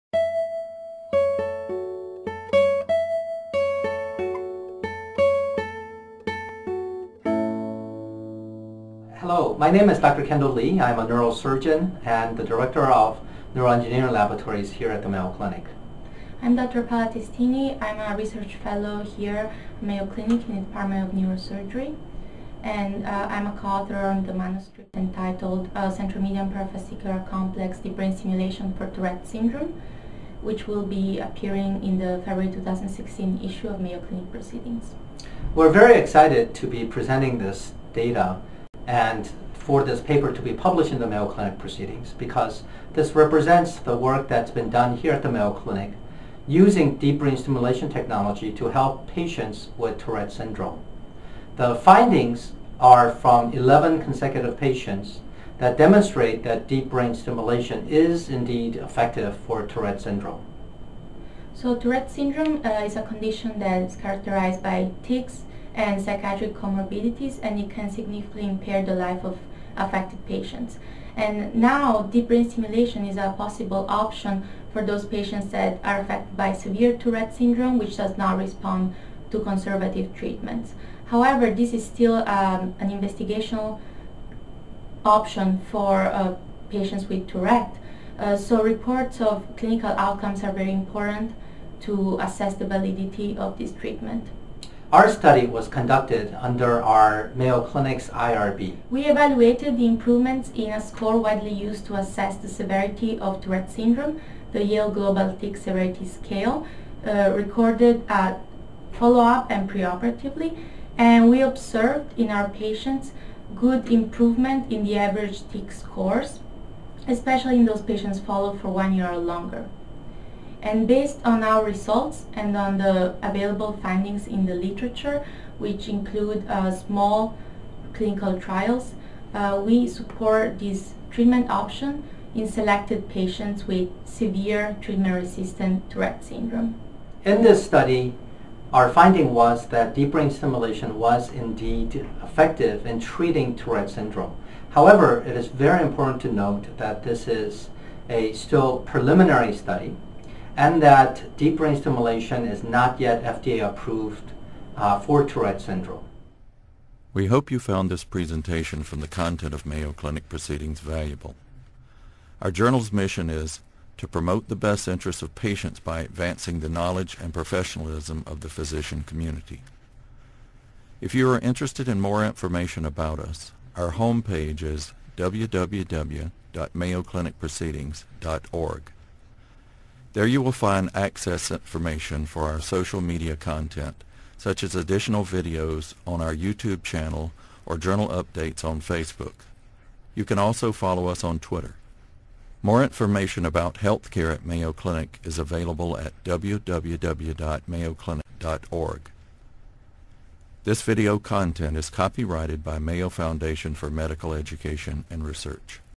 Deep Brain Stimulation for Tourette Syndrome
Description: Dr. Paola Testini, a Research Fellow in Neurosurgery, and Dr. Kendall Lee, a Neurosurgeon and Director of the Neuroengineering Lab at Mayo Clinic in Rochester, MN, share results of a study published in the February 2016 issue of Mayo Clinic Proceedings, which explores an effective treatment option for patients with severe Tourette Syndrome using deep brain stimulation.